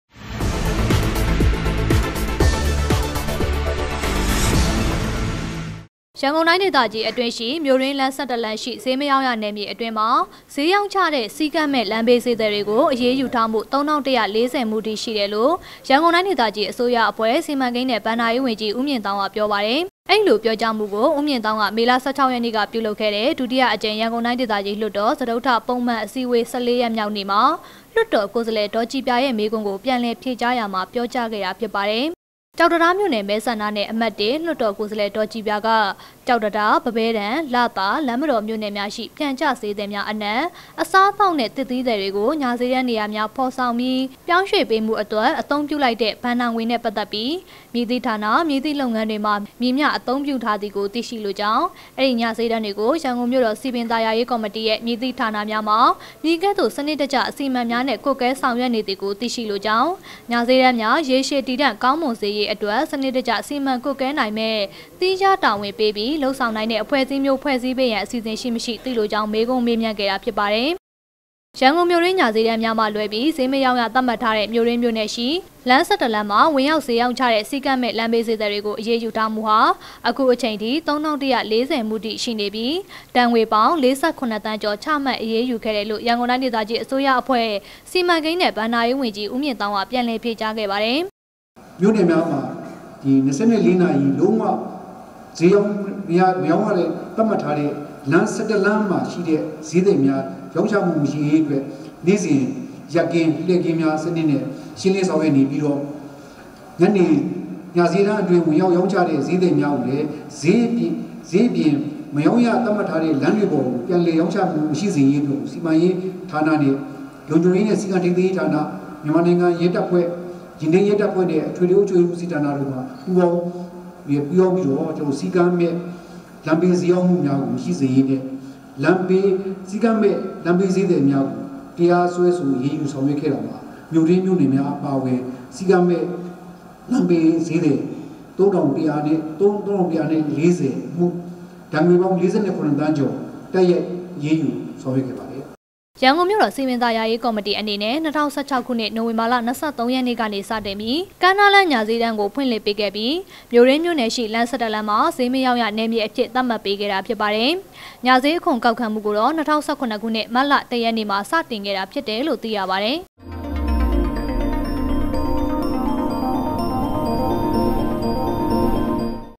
ေစ်းမေရာင္းရနယ္ေျမအတြင္းေစ်းေရာင္းခ်သည့္စည္းကမ္းမဲ့လမ္းေဘးေစ်းသည္မ်ားကို ...
ရန္ကုန္တိုင္းေဒသႀကီးအတြင္းရွိၿမိဳ႕တြင္းလမ္း၁၁လမ္းရွိေစ်းမေရာင္းရနယ္ေျမေတြ အတြင္းမွာေစ်းေရာင္းခ်တဲ႔စည္းကမ္းမဲ့လမ္းေဘးေစ်းသည္ေတြကိုအေရးယူထားမႈ၃၁၄၀မႈ ရွိတယ္လို႔ရန္ကုန္တိုင္း ေဒသႀကီးအစိုးရအဖြဲ႕စီမံကိန္းႏွင့္ဘ႑ာေရး၀န္ႀကီးဦးျမင့္ေသာင္းကေျပာပါတယ္။